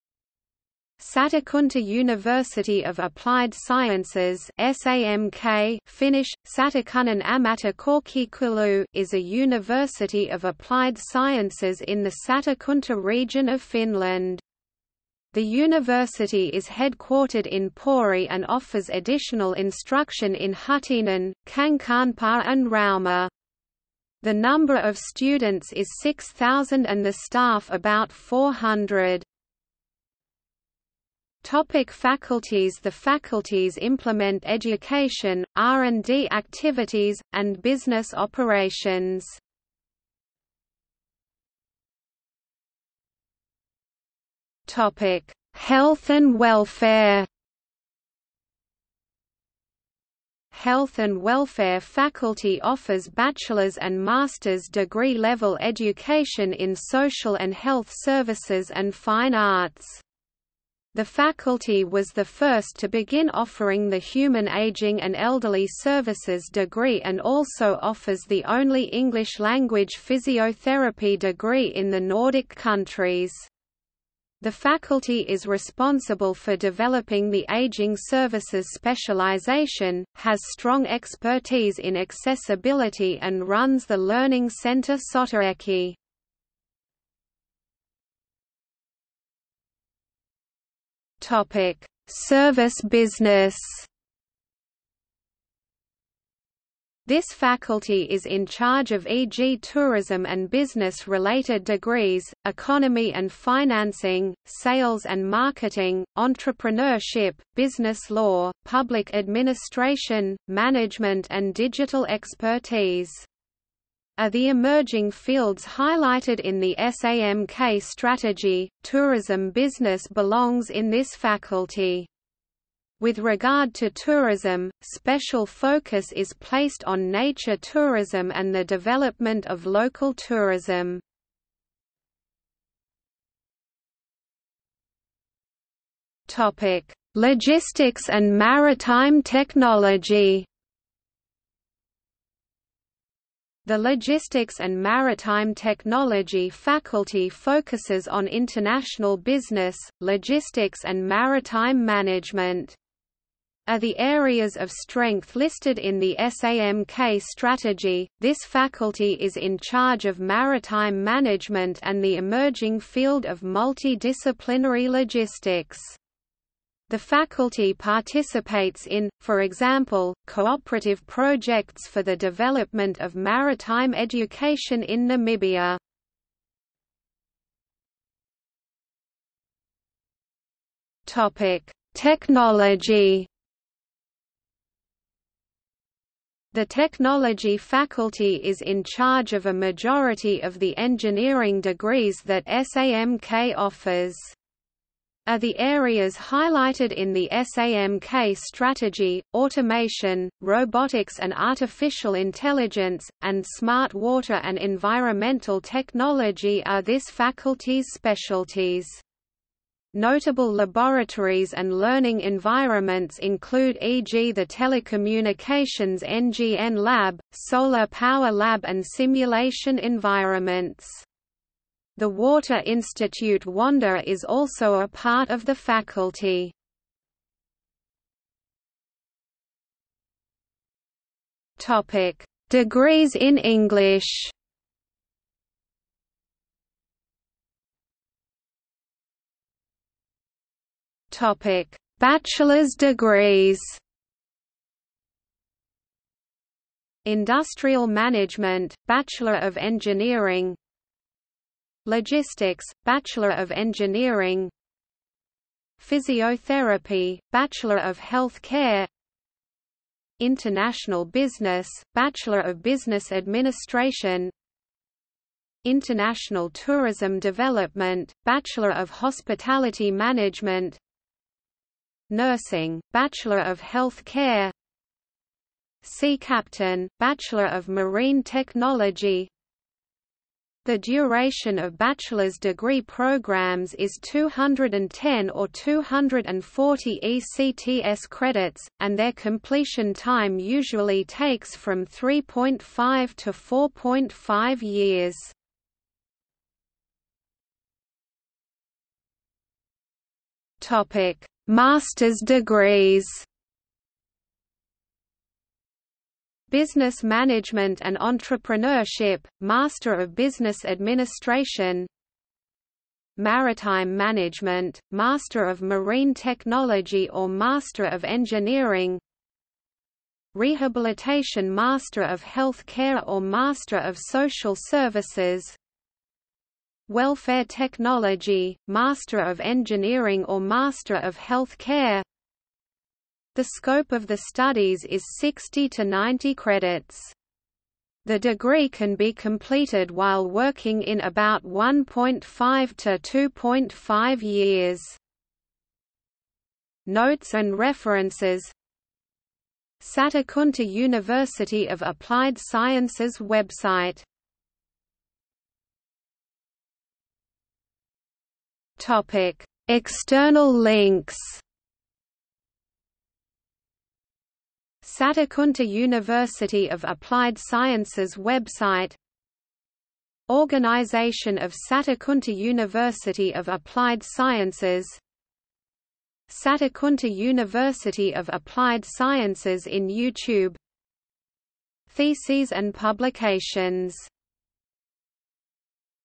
Satakunta University of Applied Sciences | Wikipedia audio article
00:00:31 1 Faculties
00:00:41 1.1 Health and Welfare
00:01:28 1.2 Service Business
00:02:13 1.3 Logistics and Maritime Technology
00:02:57 1.4 Technology
00:03:48 2 Degrees in English
00:03:57 2.1 Bachelor's Degrees
00:04:57 2.2 Master's Degrees
00:05:55 3 External links

- Socrates

SUMMARY
Satakunta University of Applied Sciences (SAMK) (Finnish: Satakunnan ammattikorkeakoulu) is a university of applied sciences in the Satakunta region of Finland. The university is headquartered in Pori and offers additional instruction in Huittinen, Kankaanpää and Rauma. The number of students is 6,000 and the staff about 400.